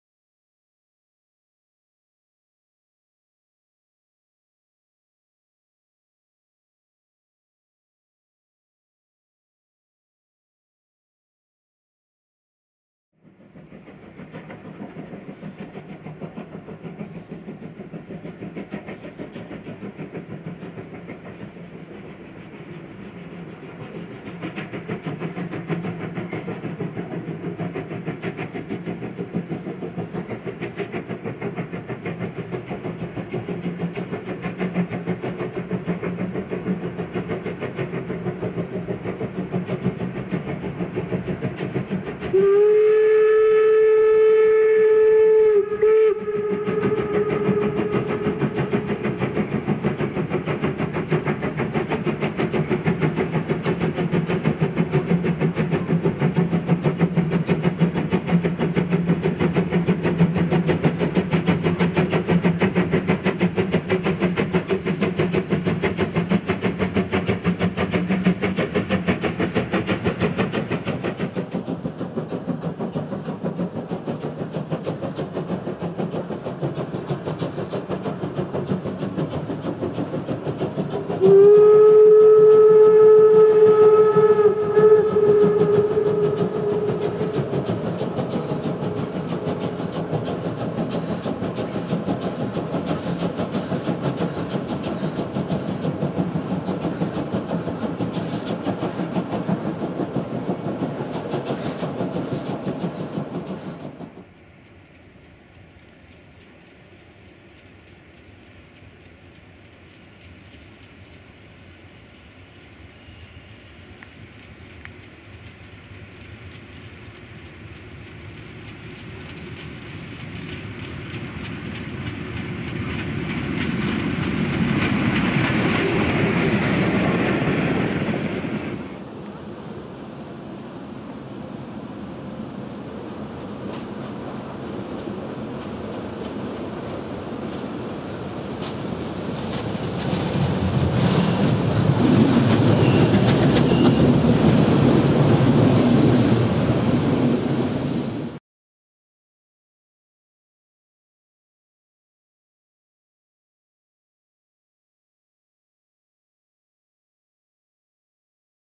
Německá parní lokomotiva 35 1097-1 - Druhý viadukt na Pražském semmeringu
Německá parní lokomotiva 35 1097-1 na Pražském semmeringu vypravená z Drážďan do Prahy u příležitosti jednodenní akce "24. Sächsisch-Böhmischer Freundschaftszug - Mit Volldampf zum Prager Weihnachtsmarkt + Prager Semmering" dne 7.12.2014.
Okružní jízda kolem Prahy jako část programu vedla ze Smíchovského nádraží po Pražském semmeringu do Hostivic a přes Rudnou zpátky Prokopským údolím zase na Smíchovské nádraží.